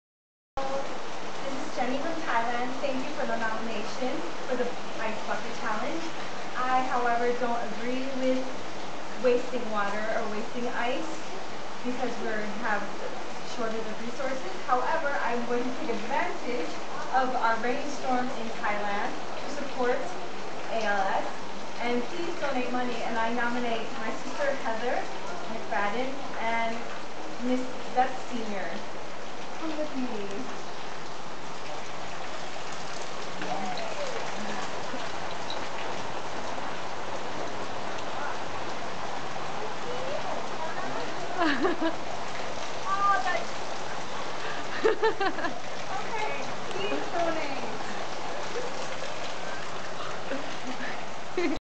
My Response to the Ice Bucket Challenge
(Sorry dark) So here we are. Everyone is doing it. For those who follow me know I do not support the ice bucket challenge for several reasons. We are wasting natural resources when there are people going without. People are blinding supporting charity without doing research or understanding the nature of giving. However, I do think it is important that if one can give one does give. So this is my 'ice bucket': the downpour rains of a Thailand rainy season storm, and roof drain (that was surprisingly freezing). Natural, using what nature has given me and nothing more. The choice to who and where you give your money is up to you. Give to something that is important to you. Spread it around. If everyone gives their money to one cause then the majority of people who need help will not get help because everyone is following the sheep in front of them. When you do give give from you heart and MIND. It is important that you know what you doing and for whom. Where is your money actually going? How much will your money actually helping the cause. And try not to waste as much of our earth as possible while doing so. When you give it should be in a way that does not take from somewhere else or depleat what we need. Thank you for giving me a moment to share my (not so interesting) ideas with you.